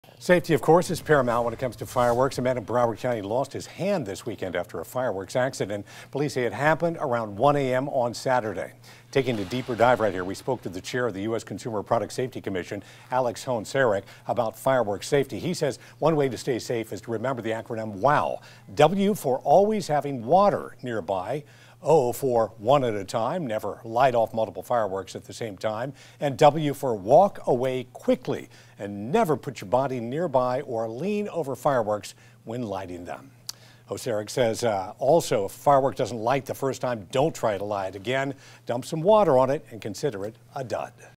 Broward man blows off hand in 4th of July weekend fireworks mishap
A Florida man lost his hand in an Independence Day weekend "fireworks accident," CBS News reports.

It happened around 1 a.m. Saturday.

The man made his way to a hospital in a private car — without his hand.